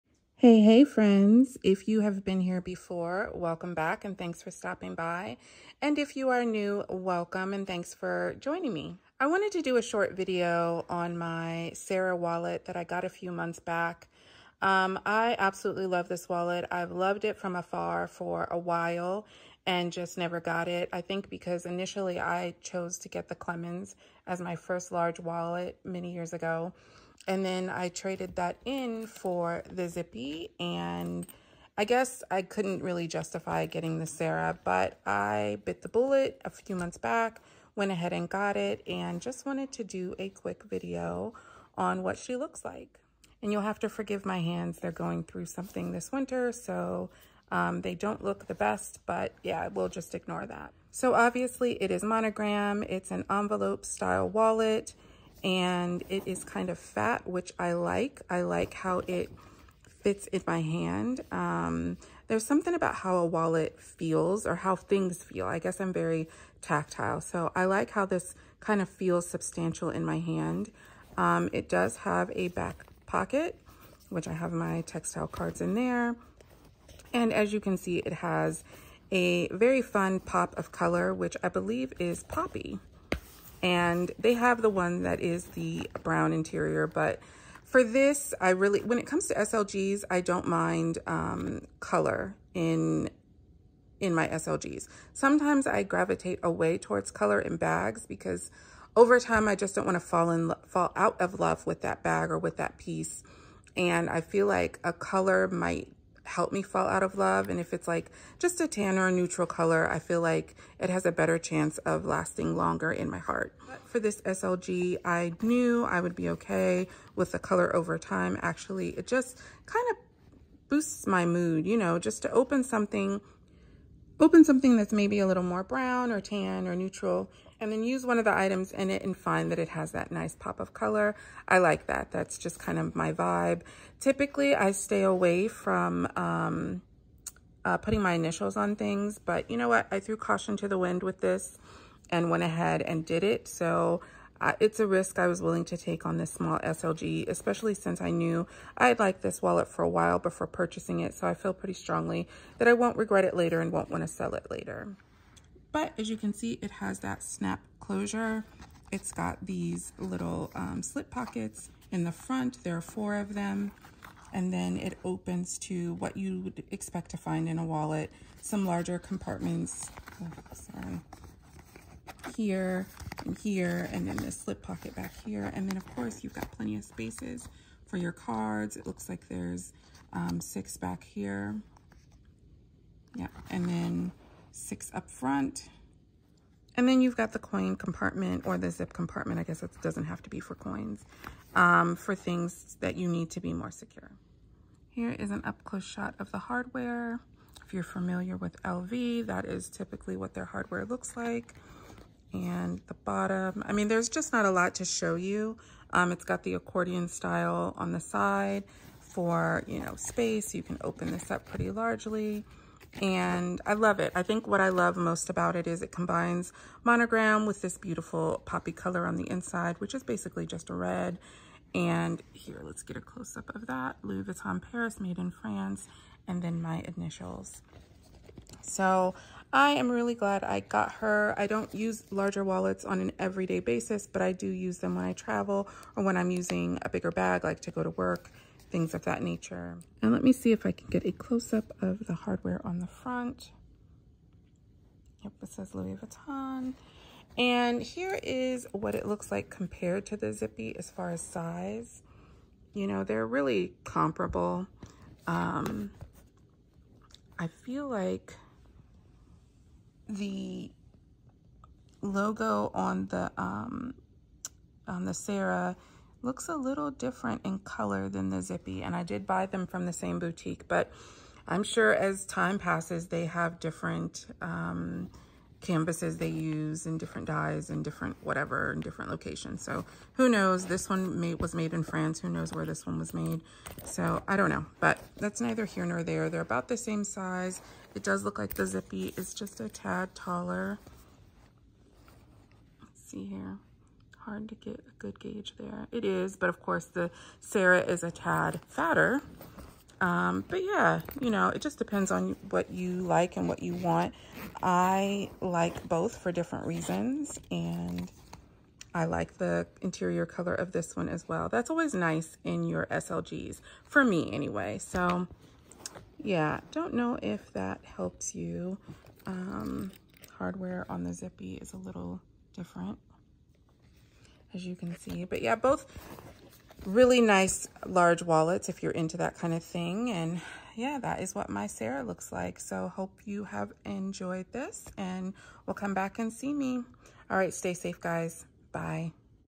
LV | Sarah Wallet
Just a short little show and tell on my Louis Vuitton Sarah wallet in monogram.